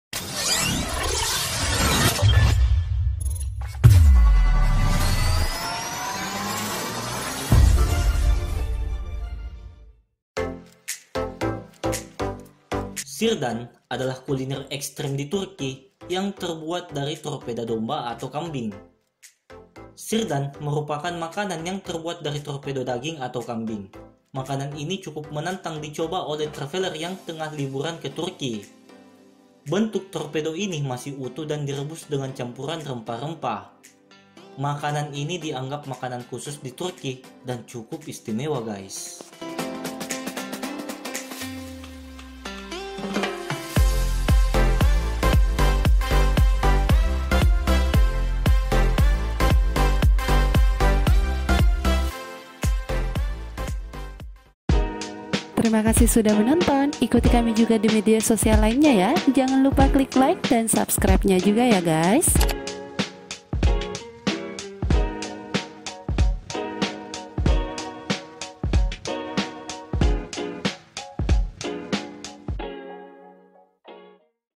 Menikmati Sirdan, Kuliner Ekstrim dari Turki yang Terbuat dari Torpedo Domba atau Kambing
Sirdan adalah kuliner ekstrem di Turki yang terbuat dari torpedo domba atau kambing.
Sirdan merupakan makanan yang terbuat dari torpedo daging atau kambing. Makanan ini cukup menantang dicoba oleh traveler yang tengah liburan ke Turki.
Bentuk torpedo ini masih utuh dan direbus dengan campuran rempah-rempah. Makanan ini dianggap makanan khusus di Turki dan cukup istimewa.